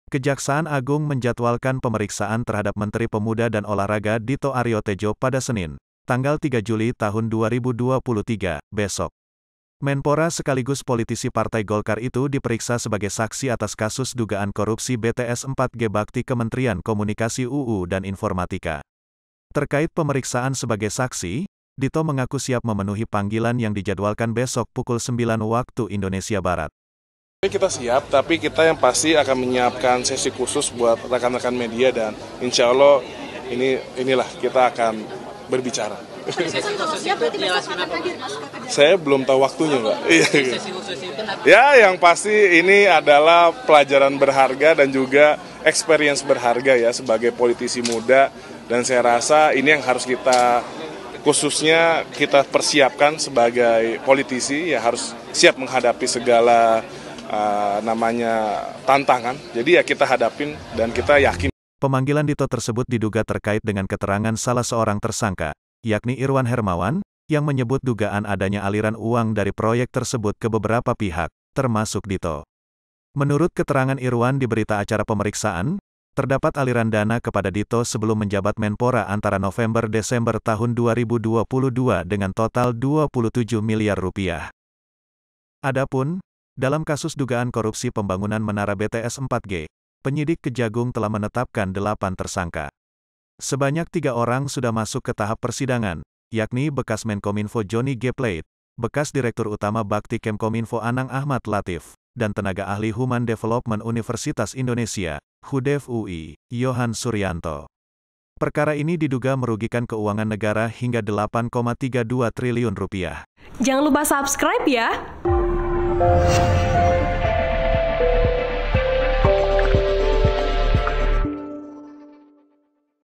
Menpora Akan Diperiksa sebagai Saksi Kasus Korupsi BTS 4G Bakti Kemenkominfo
Kejaksaan Agung menjadwalkan pemeriksaan terhadap Menteri Pemuda dan Olahraga Dito Ariotedjo pada Senin (3/7/2023), besok. Menpora sekaligus politisi Partai Golkar itu diperiksa sebagai saksi atas kasus dugaan korupsi BTS 4G Bakti Kementerian Komunikasi ÚU dan Informatika.
Terkait pemeriksaan sebagai saksi, Dito mengaku siap memenuhi panggilan yang dijadwalkan besok pukul 09.00 WIB.

Pemanggilan Dito tersebut diduga terkait dengan keterangan salah seorang tersangka, yakni Irwan Hermawan, yang menyebut dugaan adanya aliran uang dari proyek tersebut ke beberapa pihak, termasuk Dito. Menurut keterangan Irwan di berita acara pemeriksaan, terdapat aliran dana kepada Dito sebelum menjabat Menpora antara November-Desember 2022 dengan total Rp 27 miliar.

Adapun, dalam kasus dugaan korupsi pembangunan menara BTS 4G, penyidik Kejagung telah menetapkan delapan tersangka. Sebanyak tiga orang sudah masuk ke tahap persidangan, yakni bekas Menkominfo Johnny G Plate, bekas Direktur Utama Bakti Kemkominfo Anang Achmad Latief, dan Tenaga Ahli Human Development Universitas Indonesia (Hudev UI) Yohan Suryanto. Perkara ini diduga merugikan keuangan negara hingga Rp 8,32 triliun.